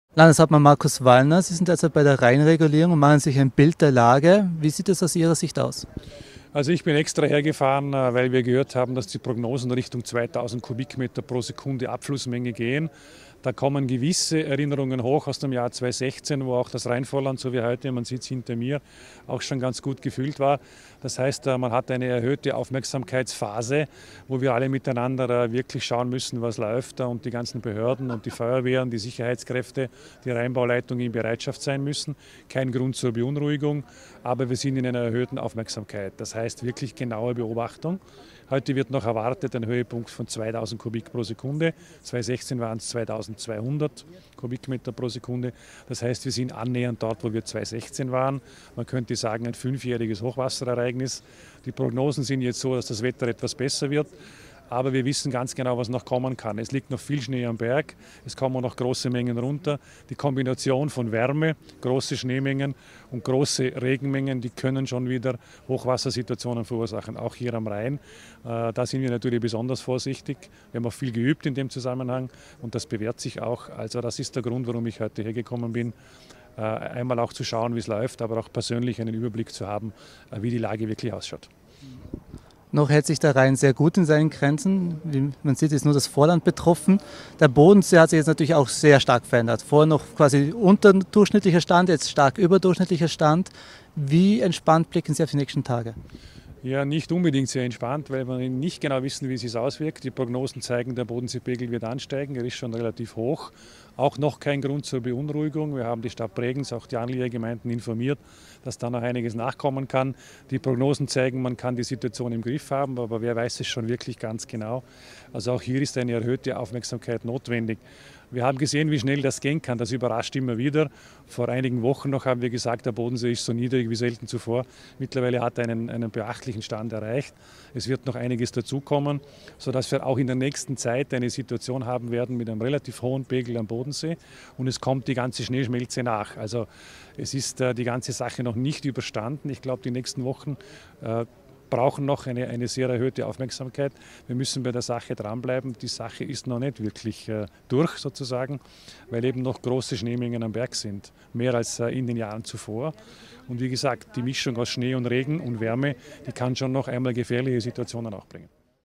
Hochwasser am Rhein: Landeshauptmann Wallner macht sich ein Bild
Am Mittwochnachmittag machte sich Landeshauptmann Markus Wallner gemeinsam mit Landesrat Christian Gantner und Bürgermeister Kurt Fischer ein Bild von der Lage am Rhein. Dieser führt seit Dienstag Hochwasser